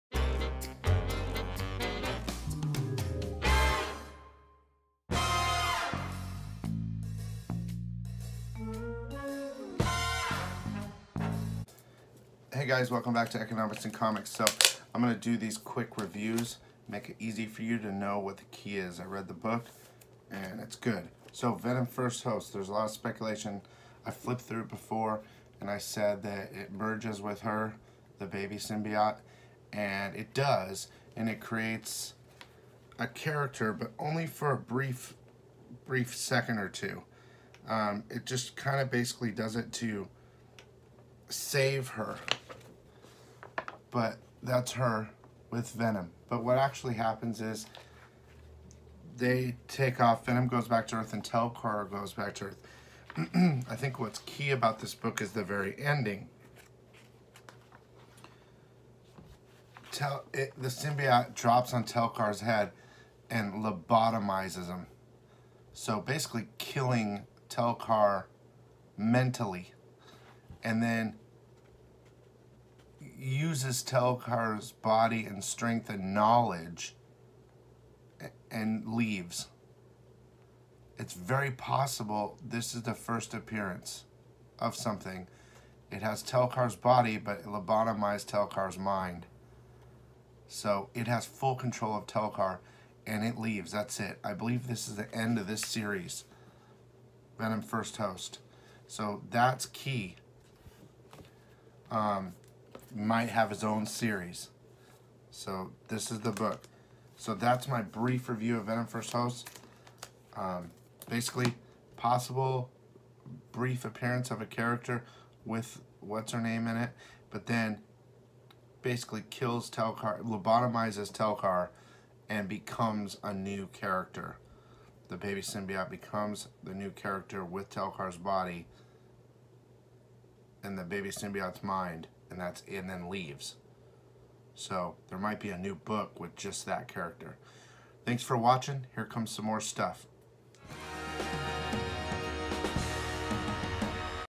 Key Comic Book Alert "Venom First Host #5" What is Key? Marvel Comics Hot 9/26/18
This video is about the hot keys available this week.  What is Key?